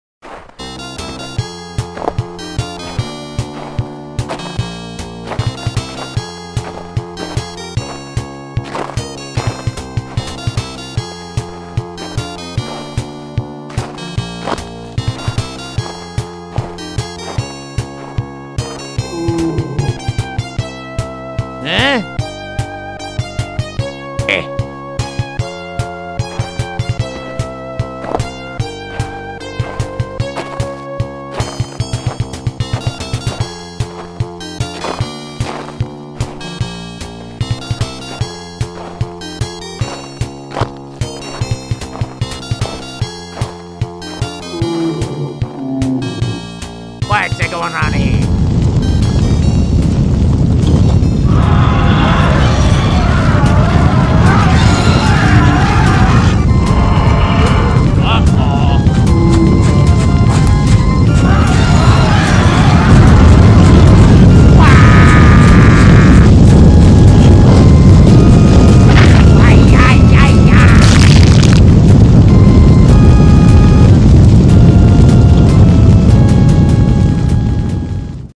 Wario gets trampled to death by a stampede of ice mammoths on Dezolis
He go squish lol